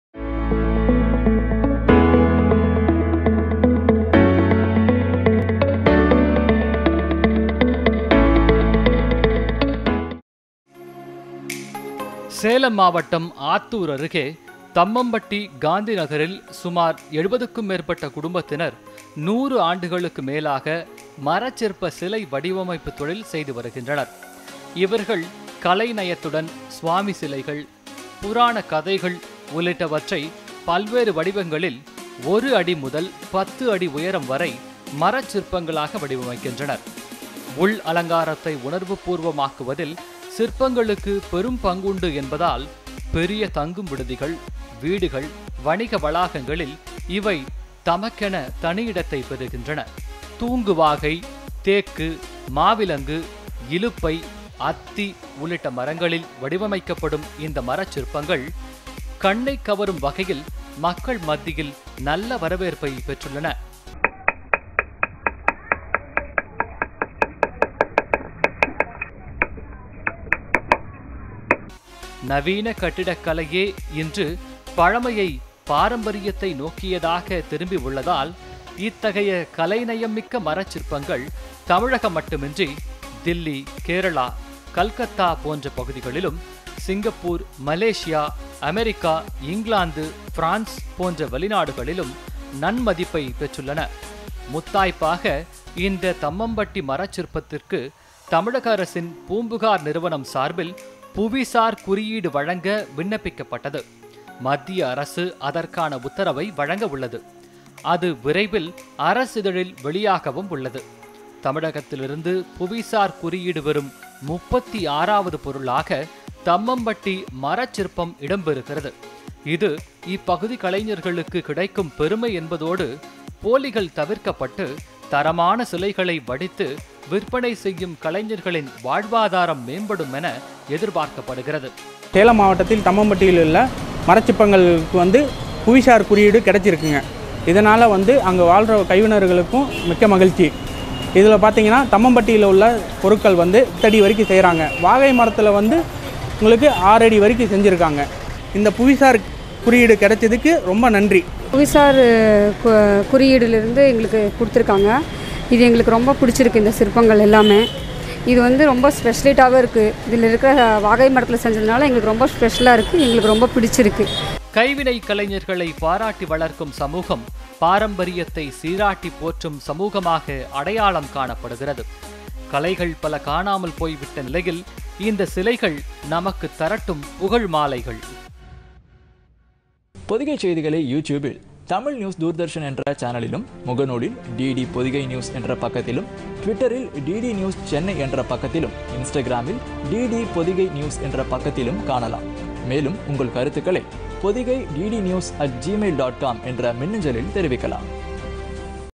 மரத்திலும் கலை வண்ணம் கண்ட கலைஞர்கள்
கல்லிலே கலை வண்ணம் கண்டான் என்ற புகழுக்குரிய தமிழகத்தில், மரத்திலும் மனம் கவர் சிற்பங்கள் படைக்கும் சேலம் மாவட்டம் தும்மம்பட்டியை சேர்ந்த கலைஞர்கள் பற்றிய செய்தி